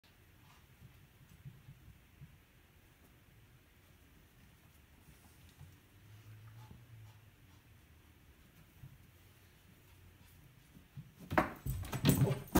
Dice falls 2017 01 28
Dice wants the cat food. Dice does not get the cat food.